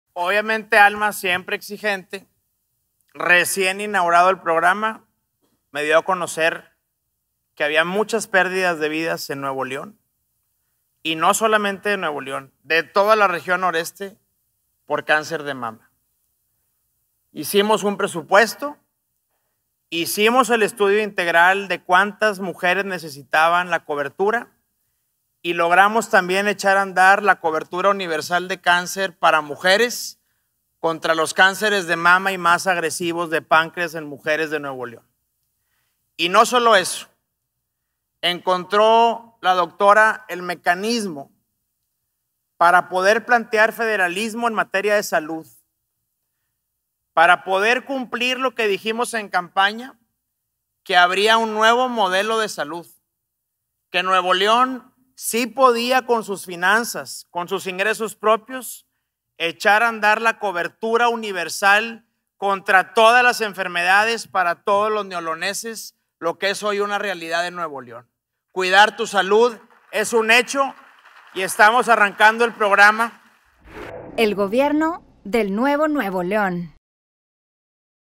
Cobertura Universal contra el cáncer de mama - Samuel García
La lucha de las mujeres contra el cáncer de mama, es una lucha en donde el Gobierno lo da todo. Por ello, creamos la CoberturaUniversal contra el cáncer de mama un programa donde podrán recibir mamografías, diagnóstico, medicamentos, tratamiento, seguimiento, prótesis y cirugía reconstructiva; incluyendo especialistas, cirujanos y psicólogos.

En Nuevo León la salud no es un privilegio, es un derecho, por eso lo damos todo.

Samuel García
Gobernador del Nuevo Nuevo León